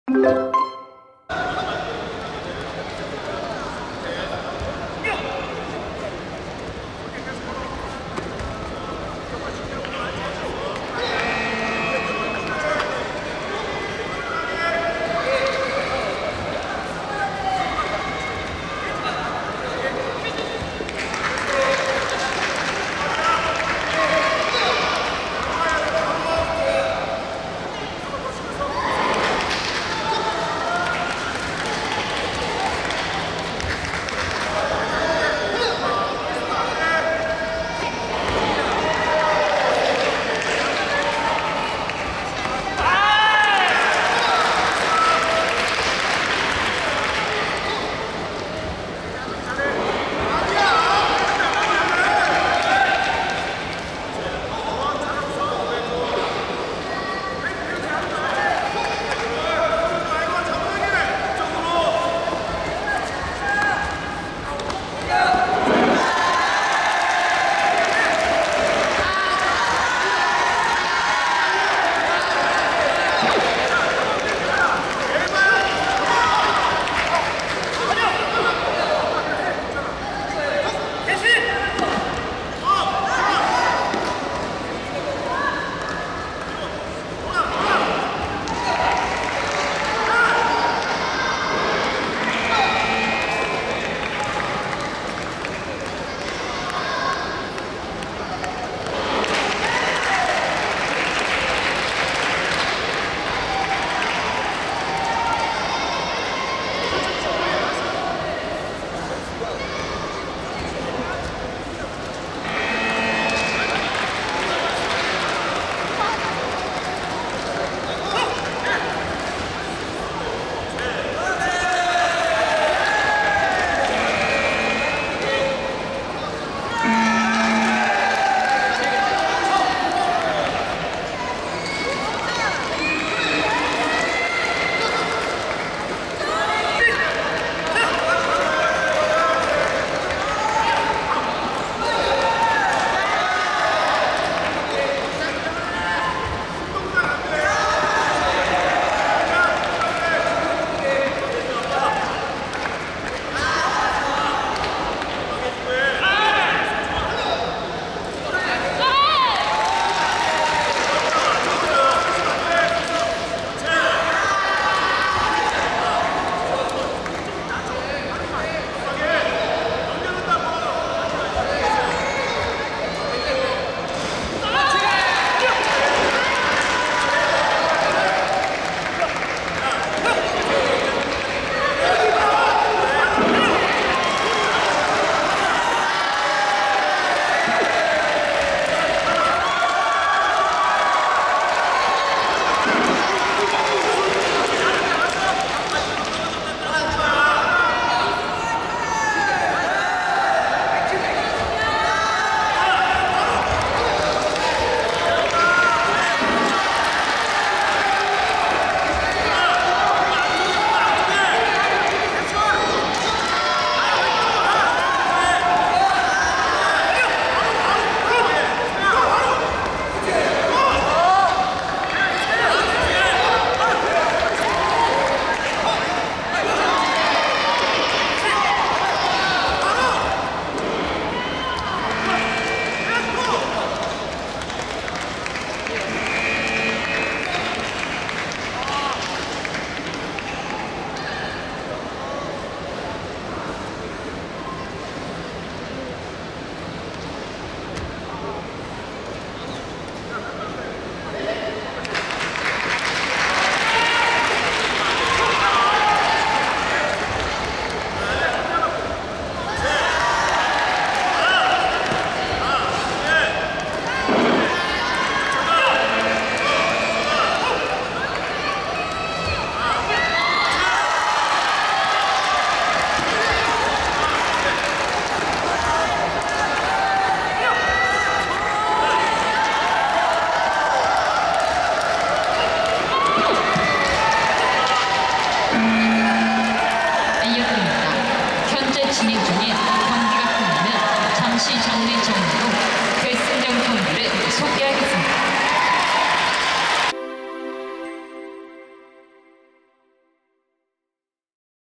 황민건(예산중) vs 문승훈(풍생중) | 준결승 남자중등부 L-웰터급 | 제53회 협회장기대회[겨루기]
ㆍ대회 : 제53회 협회장기 전국단체대항 태권도대회(겨루기)
ㆍ기간 : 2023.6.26.(월)-7.4.(화) / 9일간
ㆍ장소 : 박정희체육관 / 경북 구미
ㆍ주최 : 대한태권도협회
ㆍ주관 : 대한태권도협회, 경상북도태권도협회, 구미시태권도협회
ㆍ중계 : 대한태권도협회 유튜브 채널